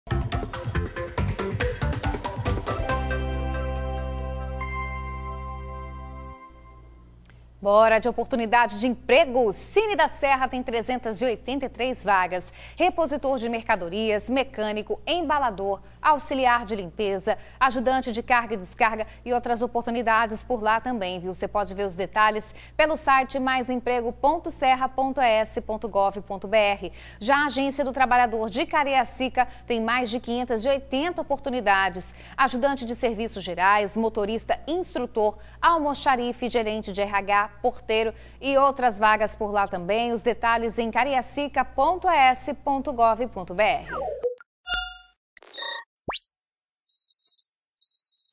964 vagas de emprego na Grande Vitória
As oportunidades estão disponíveis no Sine da Serra e na Agência do Trabalhador de Cariacica.